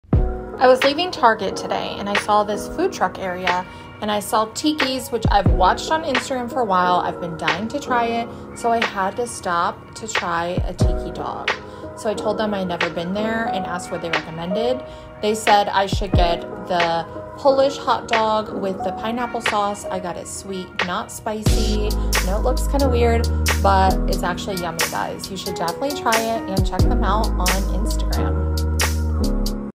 TIKI'S GOURMET HAWAIIAN HOT DOGS 😋
Gourmet Hawaiian Hot Dog food truck in Las Vegas.